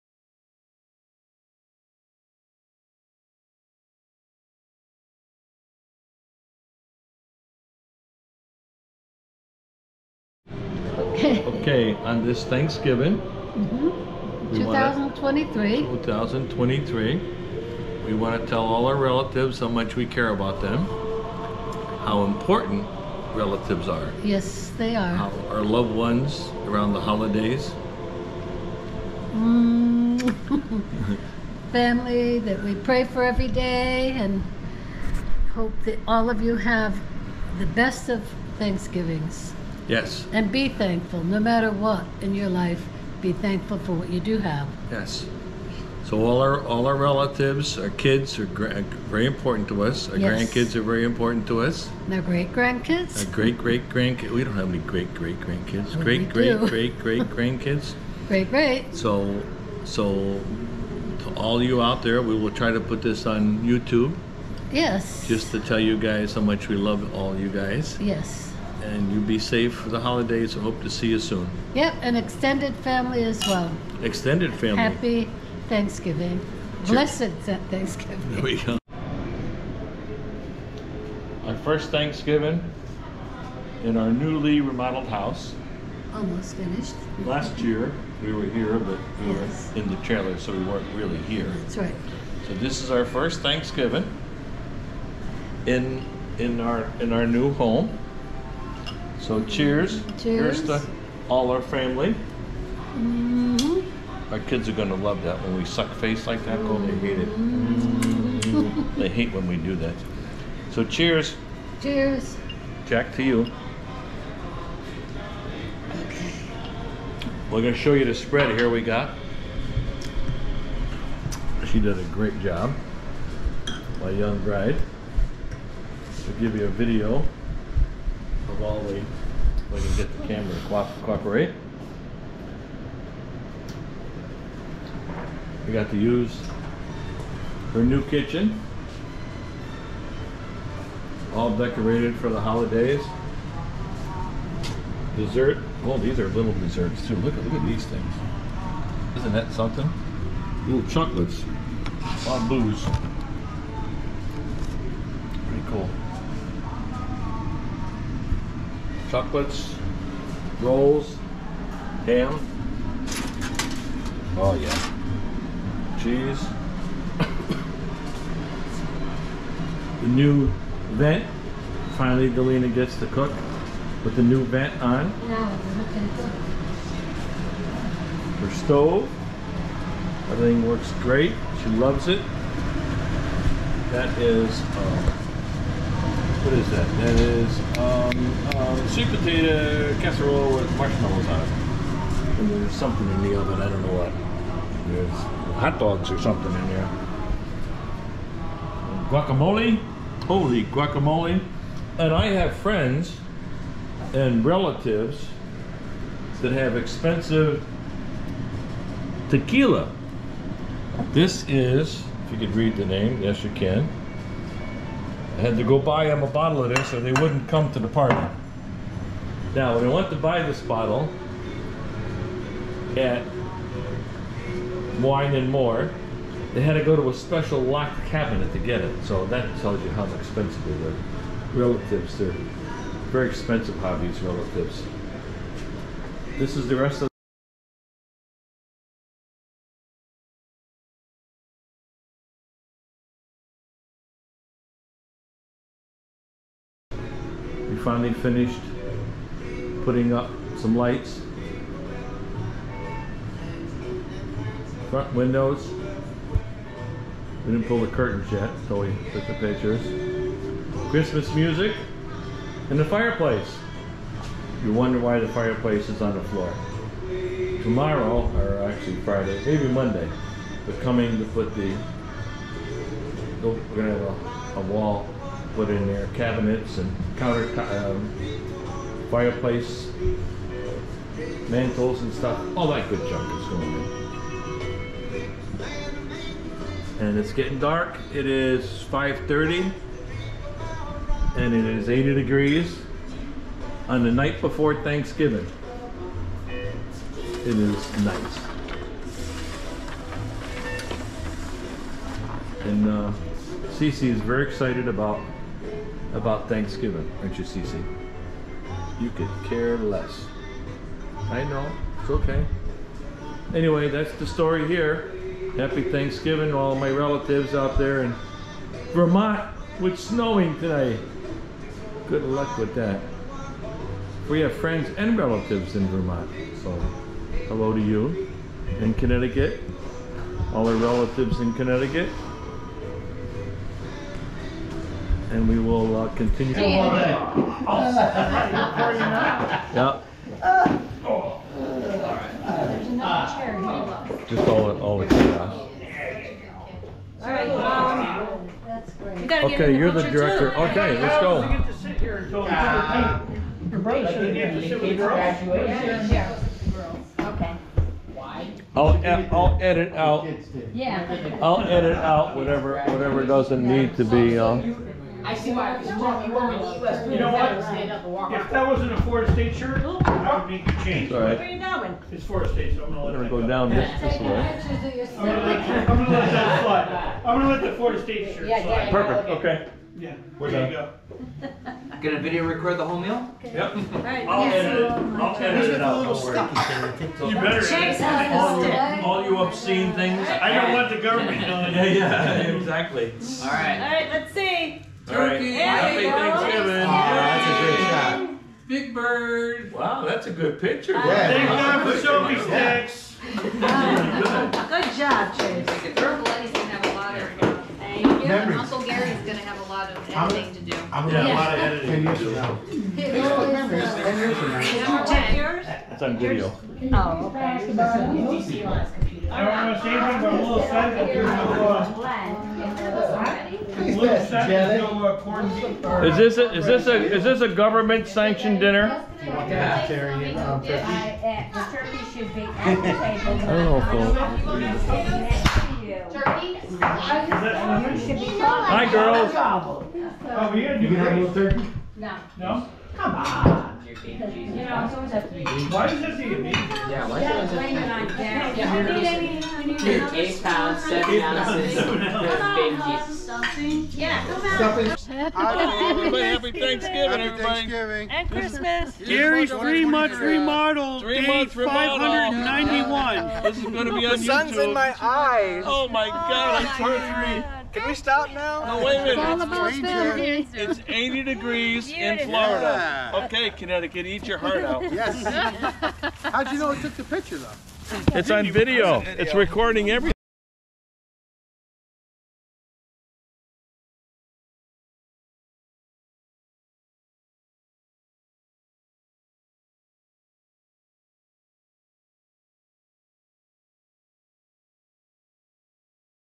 Thanks Giving 2023
Thanks Giving in our newly Three Month Remodeled.
That actually took year and half.